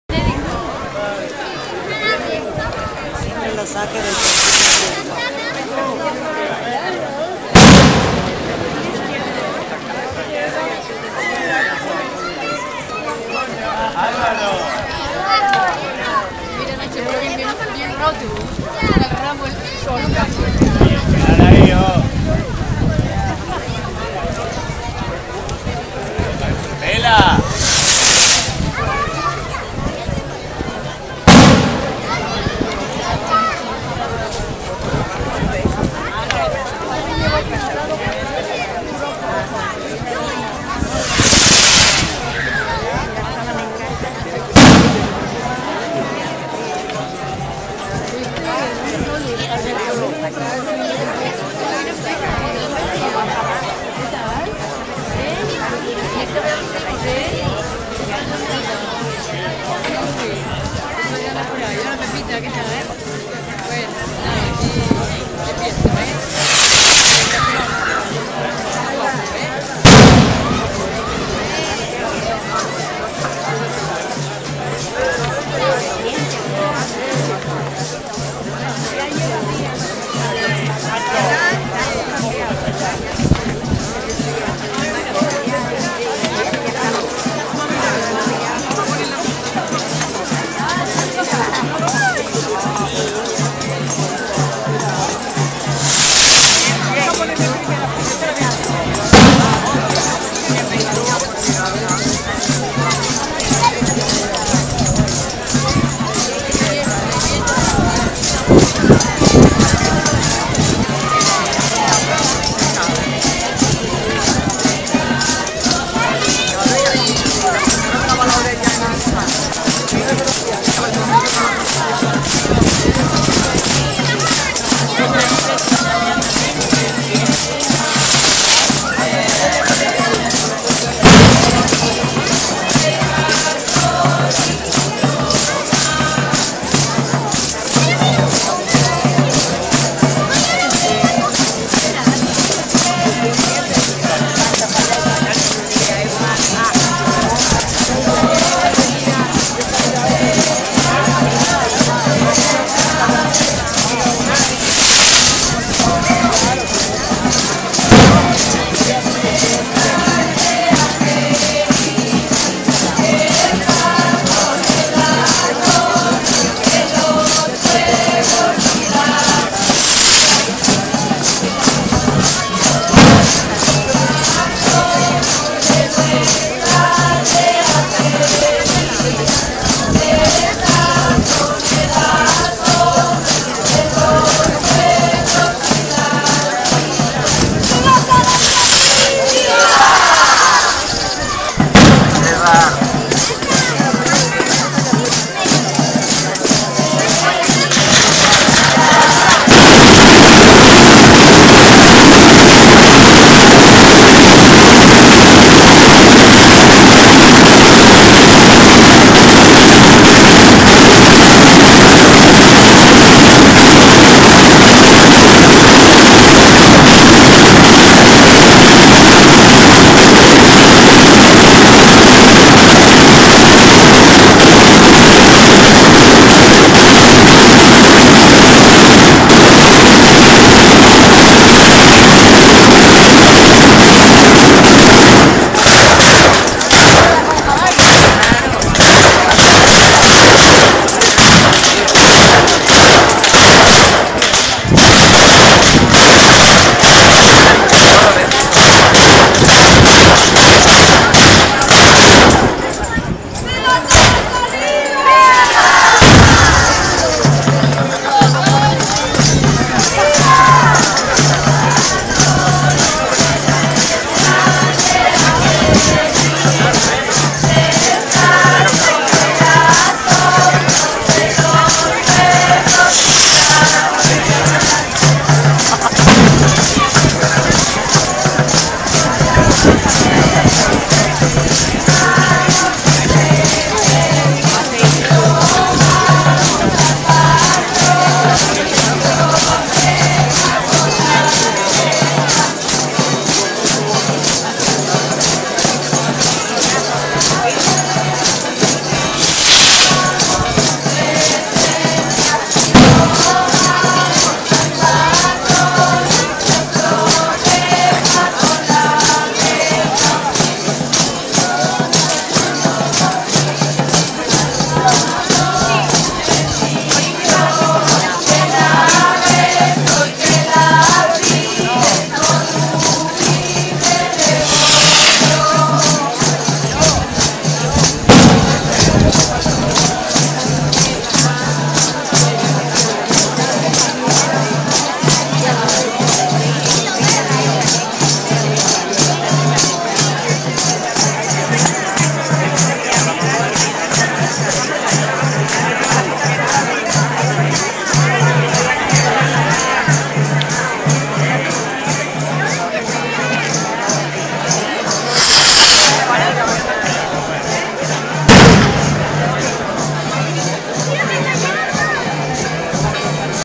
Naves de Llanes. Procesion de San Antolin 2013
Procesion de San Antolin durante las fiestas el 2 de Septiembre del 2013 en Naves de Llanes, Asturias.